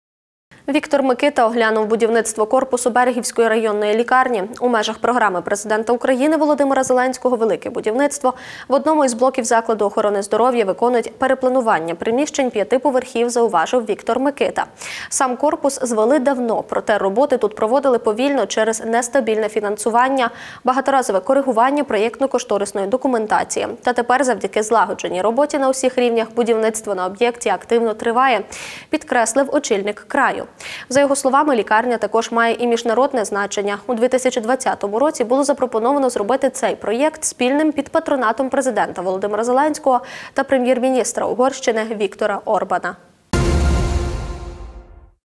Оглянули будівництво
Віктор Микита оглянув будівництво корпусу Берегівської районної лікарні. «У межах програми Президента України Володимира Зеленського «Велике будівництво» в одному з блоків закладу охорони здоров'я виконують перепланування приміщень п'яти поверхів», — зауважив Віктор Микита. Сам корпус звели давно, проте роботи тут проводили повільно через нестабільне фінансування, багаторазове коригування проєктно-кошторисної документації. «Та тепер завдяки злагодженій роботі на усіх рівнях будівництво на об'єкті активно триває», — підкреслив очільник краю. За його словами, лікарня також має й міжнародне значення. У 2020 році було запропоновано зробити цей проєкт спільним під патронатом Президента Володимира Зеленського та Прем'єр-міністра Угорщини Віктора Орбана